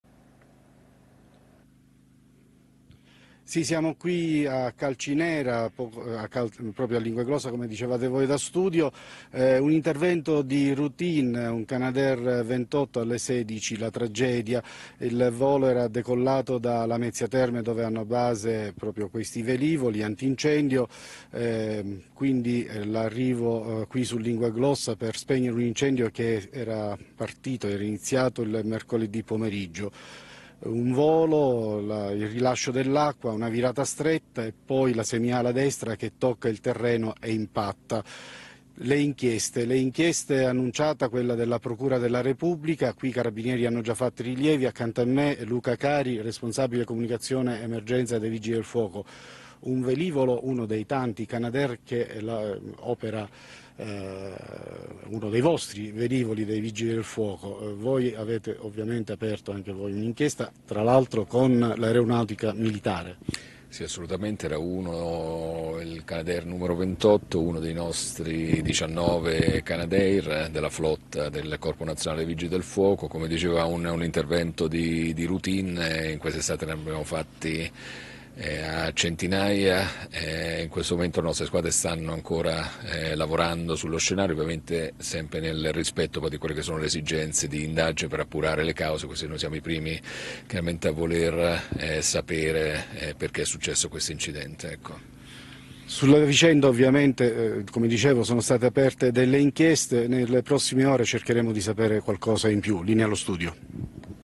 Catania, Canadair dei Vigili del fuoco precipita sulle pendici dell'Etna
Fonte: RAI NEWS 24 CRONACHE DALL'ITALIA